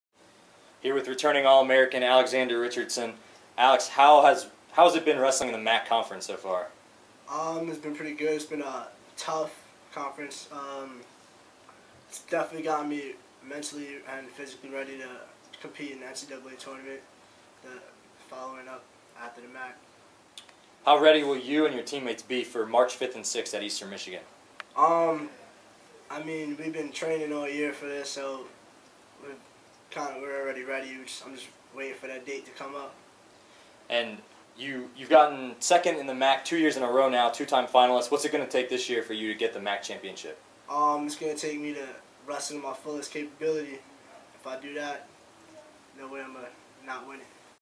Lenny MACs
"If I wrestle to my full potential... No way I'm not winning it." - Alexander Richardson ready for the MAC Championships.